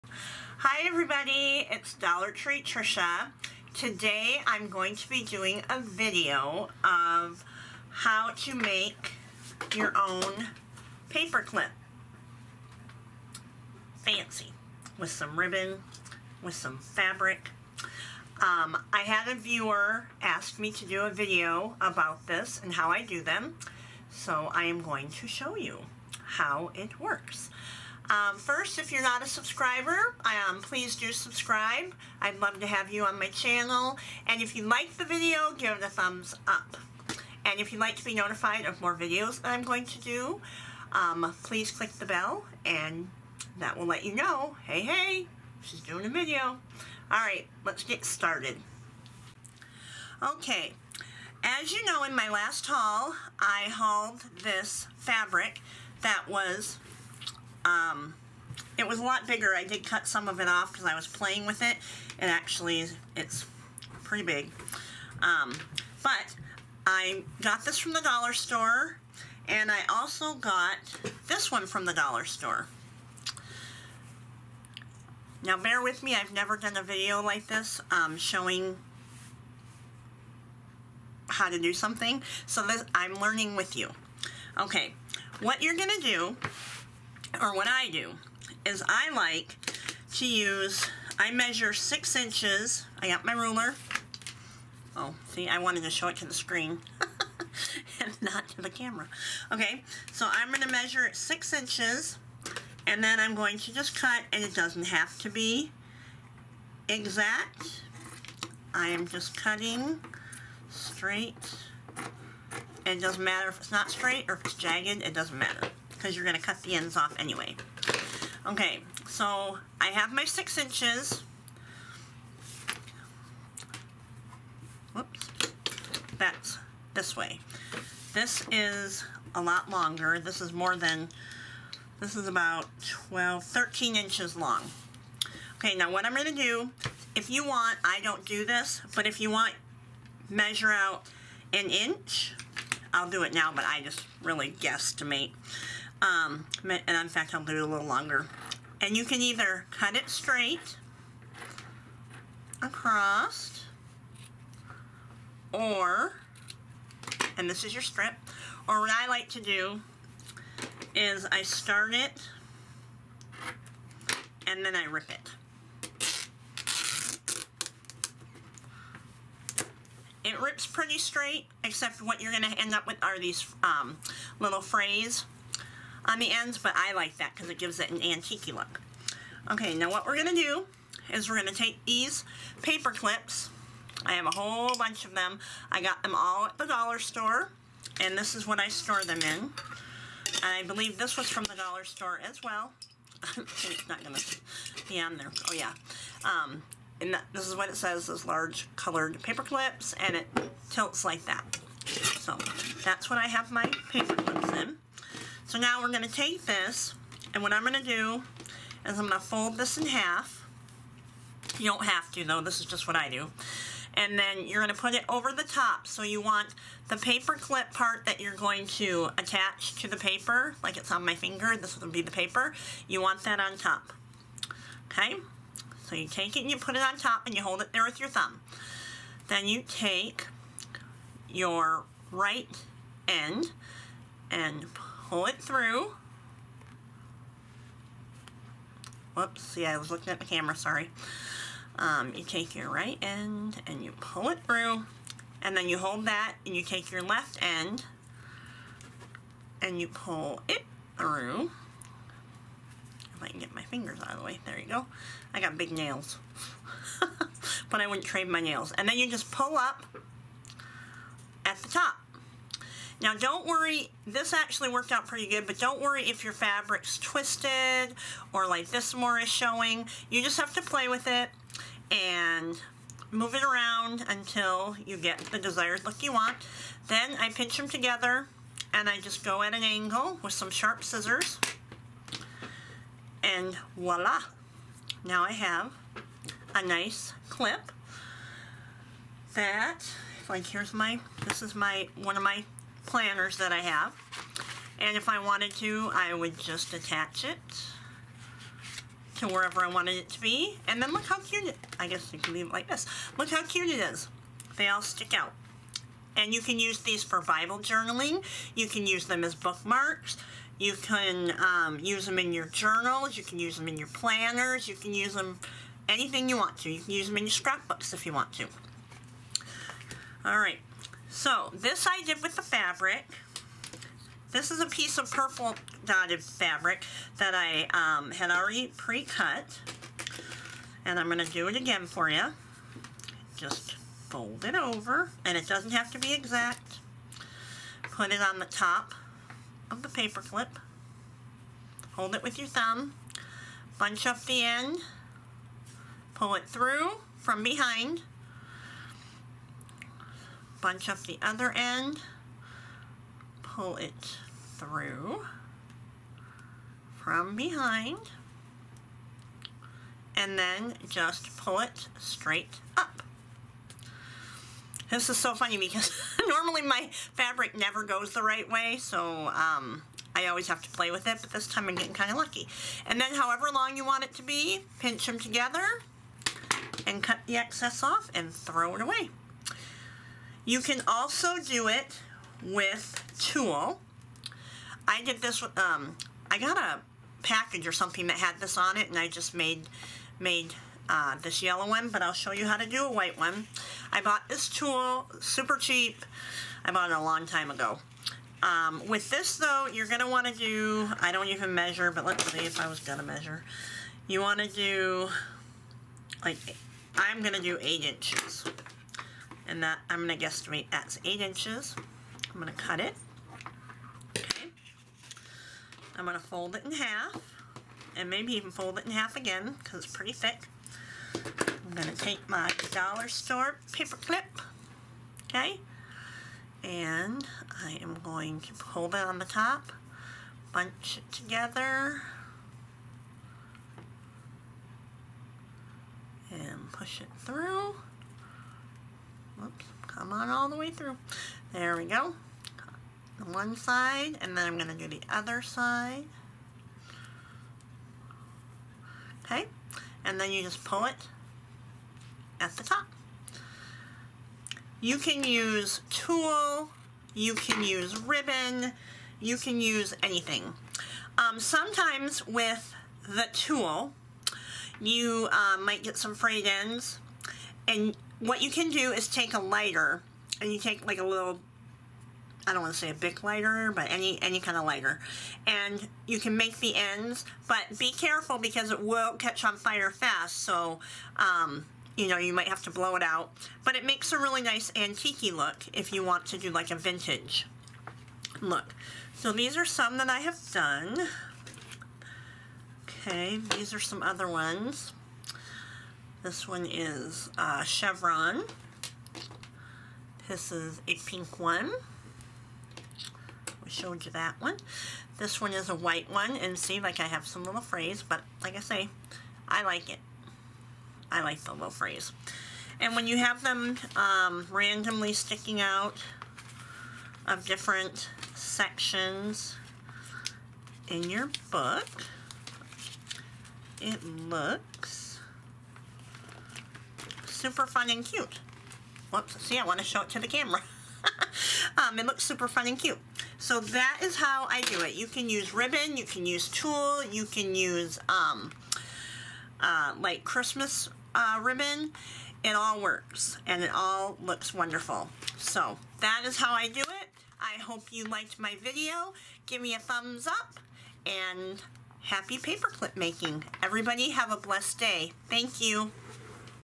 Dollar Tree Tricia Ribbon Paperclip Tutorial - July 24 2019
Follow along as I show you how easy it is to make a fabric paperclip.